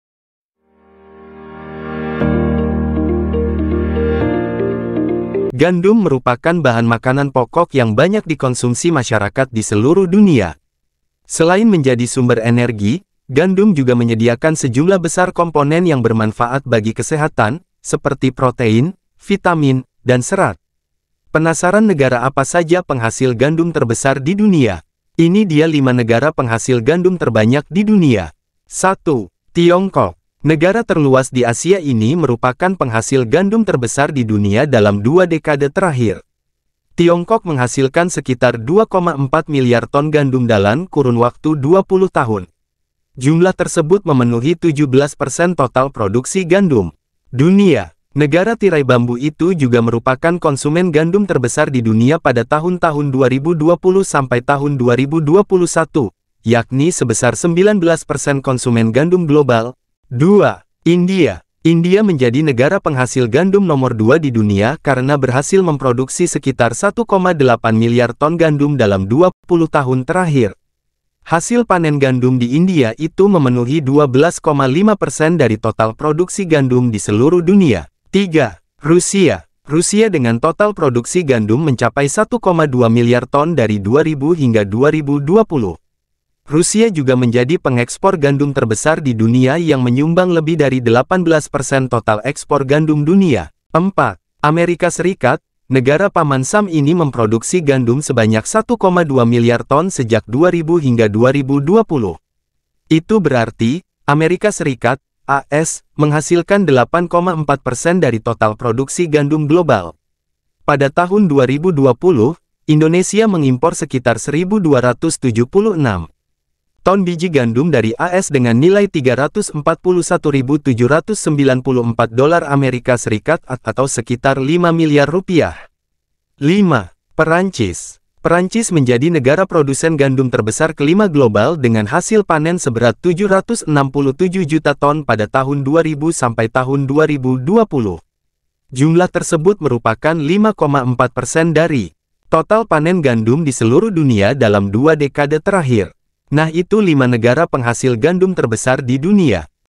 5 Negara Penghasil Gandum Terbesar Di Dunia
5 Negara Penghasil Gandum Terbesar Di Dunia
Gandum merupakan bahan makanan pokok yang banyak dikonsumsi masyarakat di seluruh dunia. Selain menjadi sumber energi, gandum juga menyediakan sejumlah besar komponen yang bermanfaat bagi kesehatan, seperti protein, vitamin, dan serat.